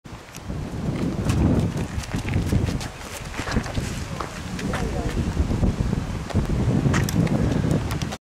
Boot-throwing by Ville Huuskonen 12th June 2010
National team qualification for World Championships 2010 was organized in Tuusula 12th June 2010.